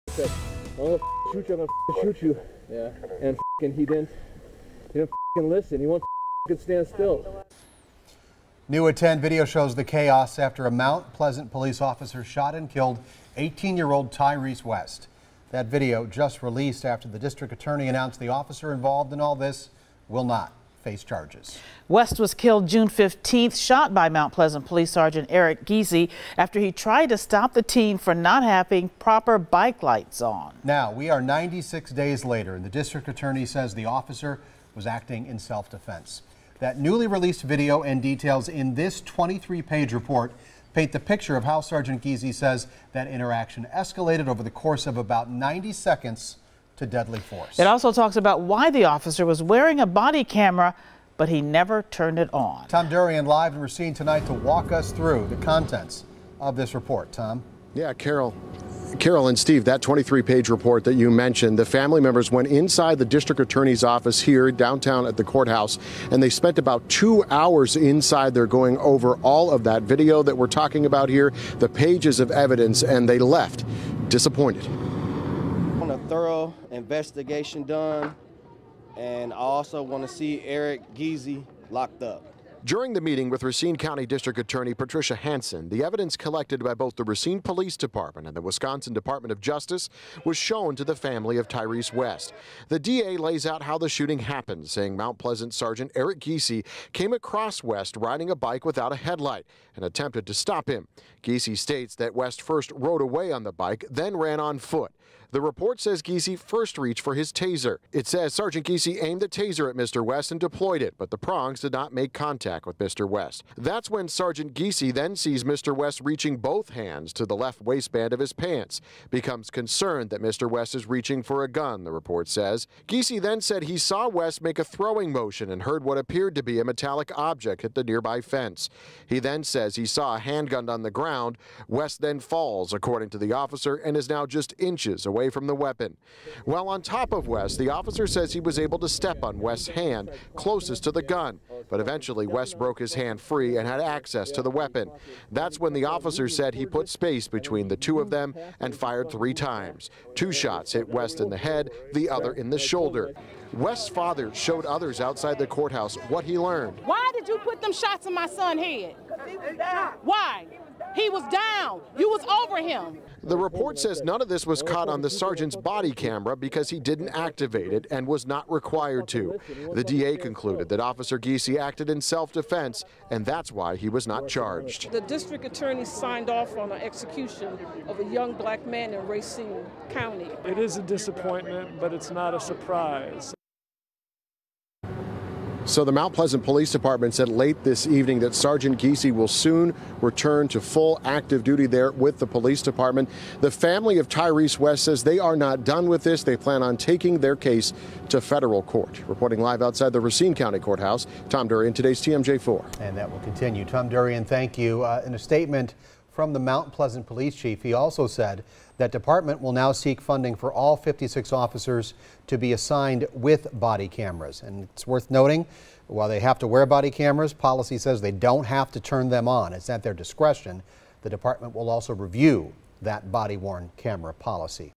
Police sergeant will not face charges in shooting death of Ty'Rese West
The Racine County District Attorney released a full report behind the decision not to charge Police Sgt. Eric Giese in the shooting of Ty'Rese West.